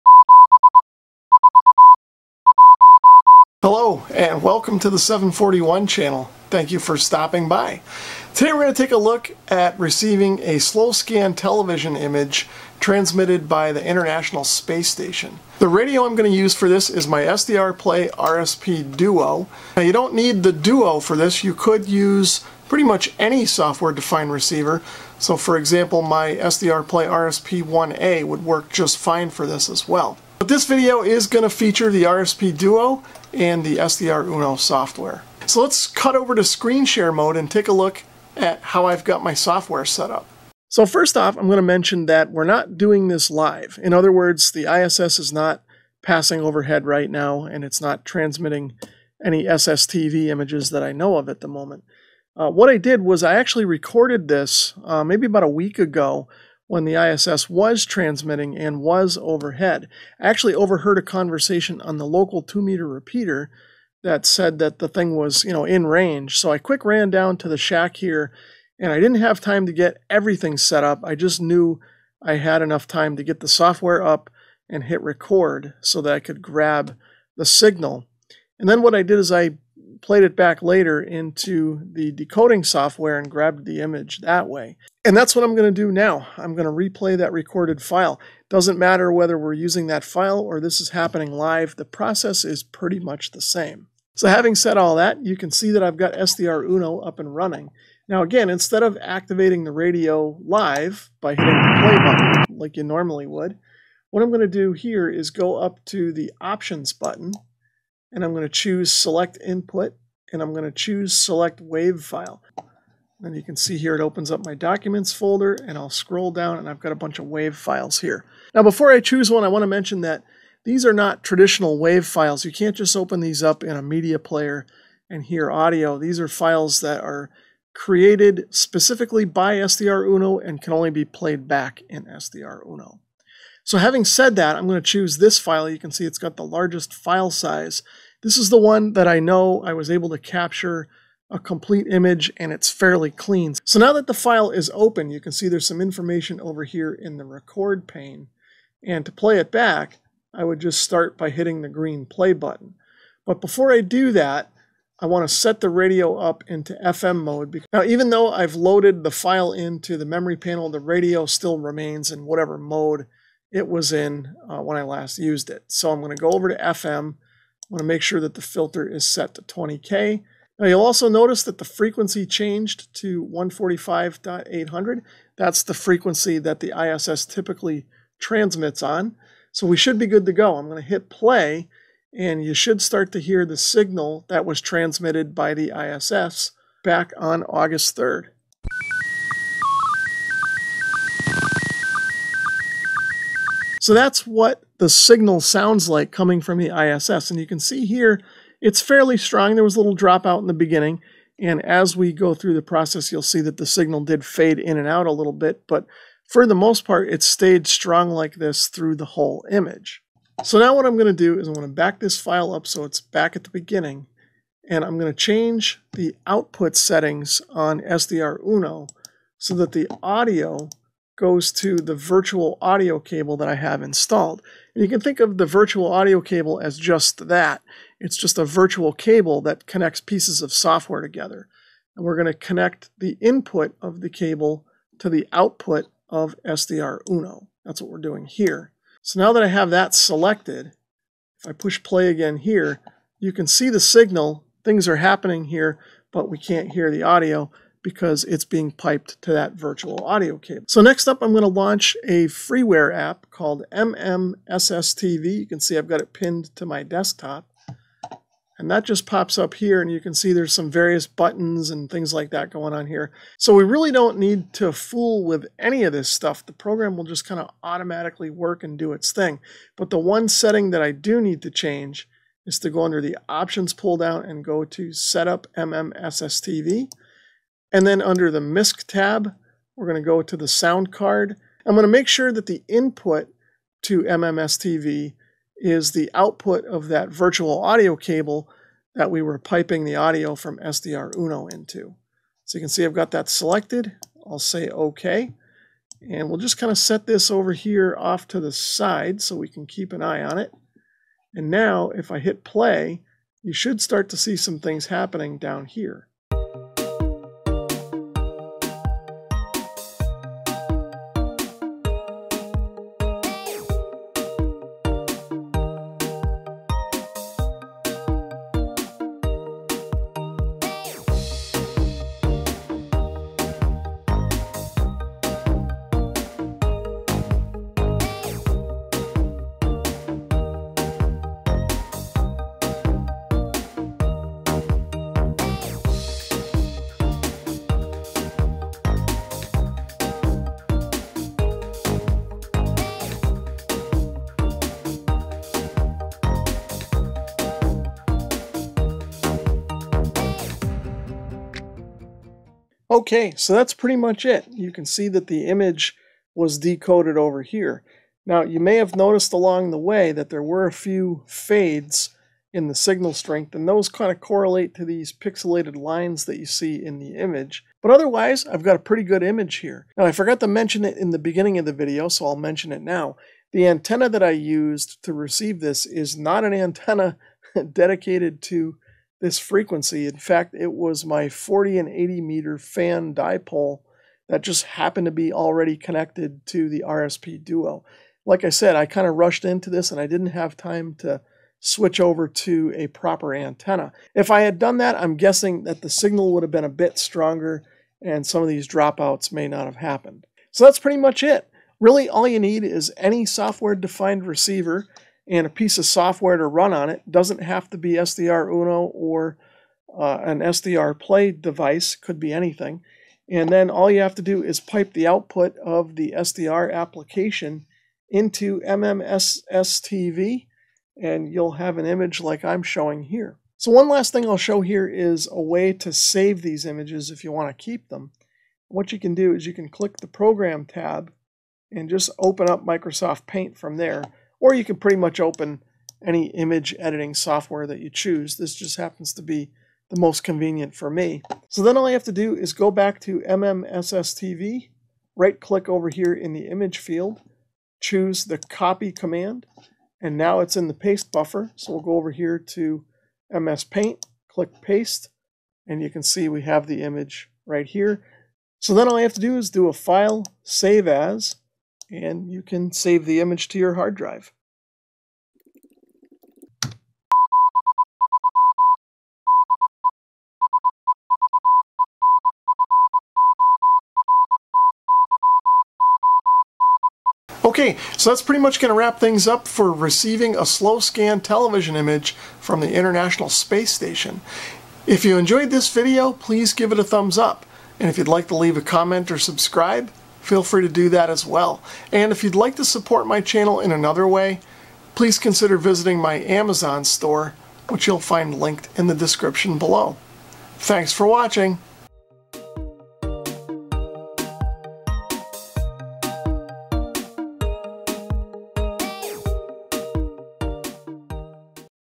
How to receive an image from the Space Station (ISS) using an SDRplay radio
In this video I show how I set up my SDRplay RSPduo to receive a SSTV image from the international space station using SDRuno and MMSSTV software